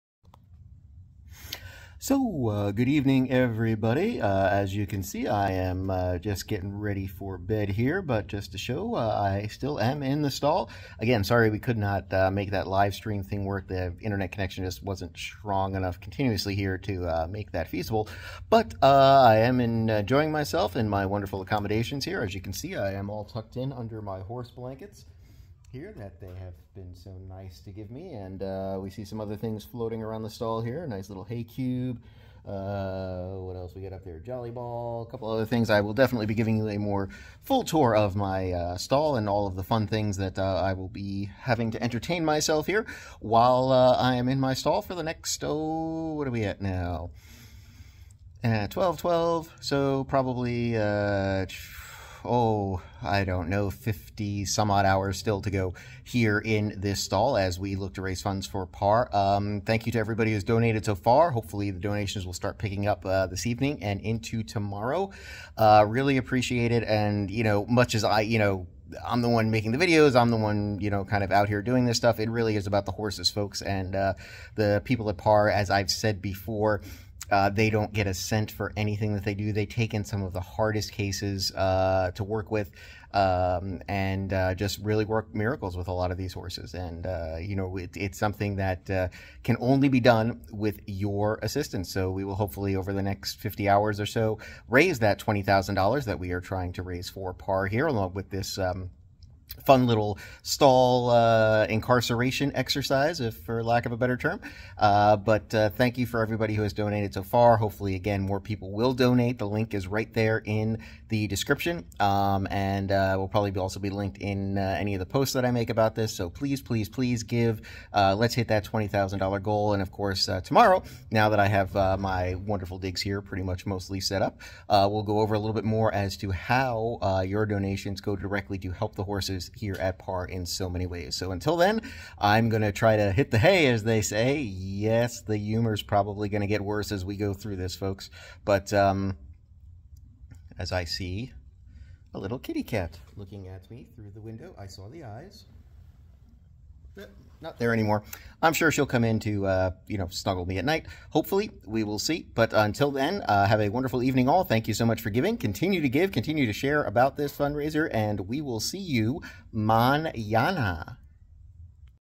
Goodnight from "Doc" as he Enters Night #1 of the 20k fundraiser for PARR OTTB Rescue
I get settled in for the first night of my stall confinement here at the PARR OTTB Rescue facility here in Ocala after setting up my stall for the next 72 hours of life. Thank you to all who have donated so far and can't wait to see what tomorrow may bring as we journey onward...